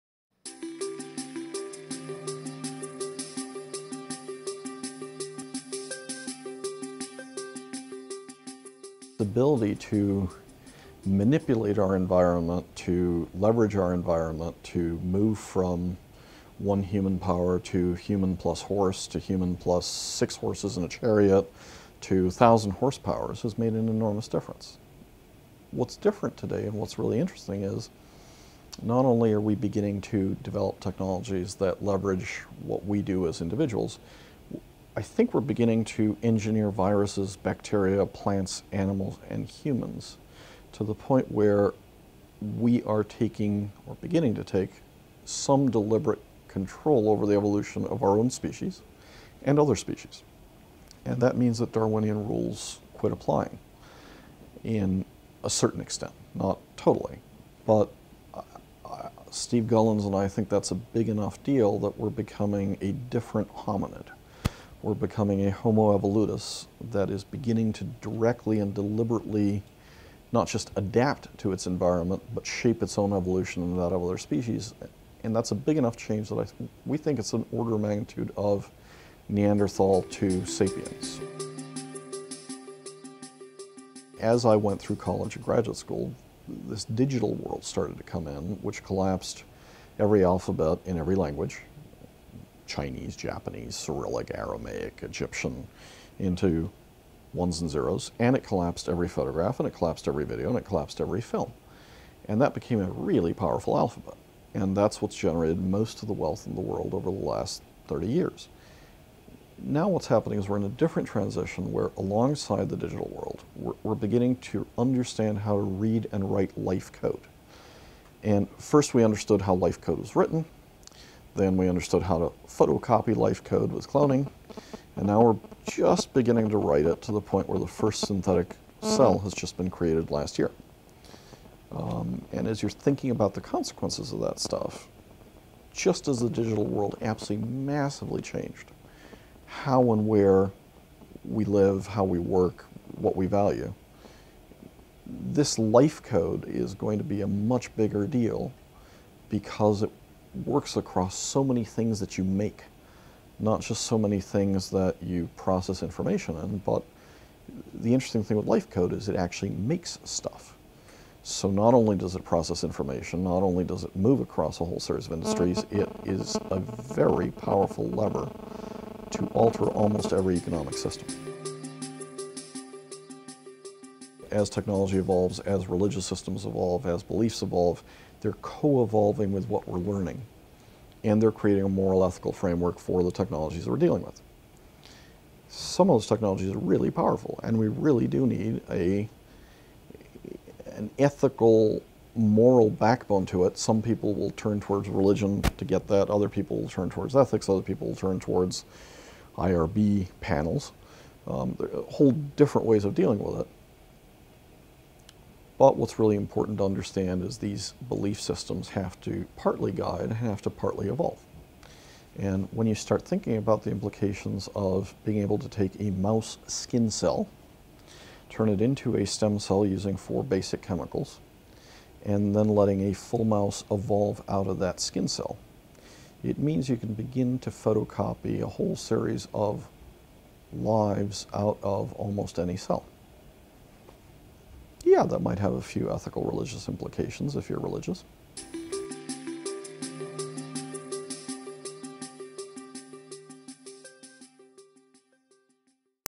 Juan Enriquez Interview.Life Sciences.mov
VC and Former Founding Director, Life Sciences Project at Harvard Business School discusses the implications of the Life Sciences revolution.